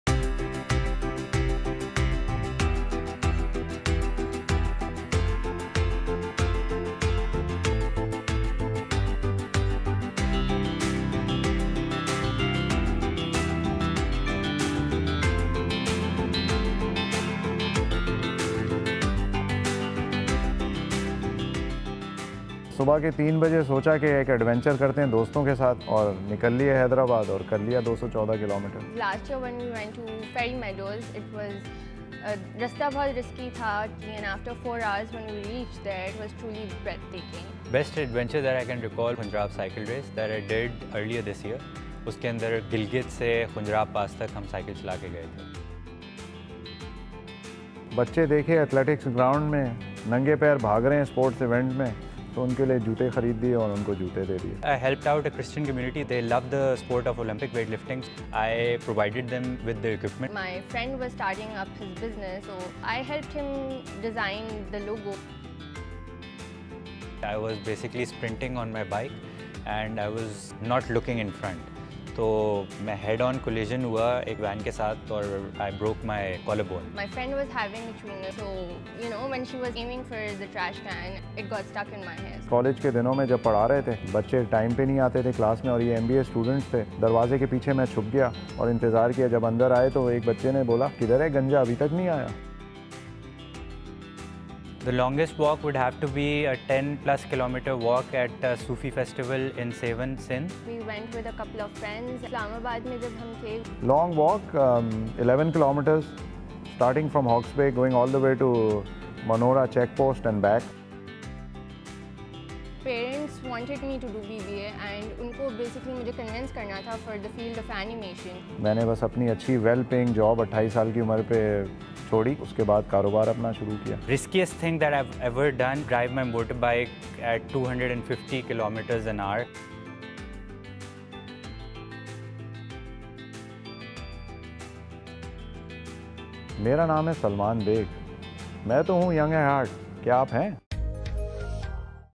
Be Young At Heart | Kia Pakistan
Being Young at Heart is not just a philosophy but a way to lead life. Witness these extraordinary individuals who lead their lives the youthful way in our first episode of the Young At Heart series.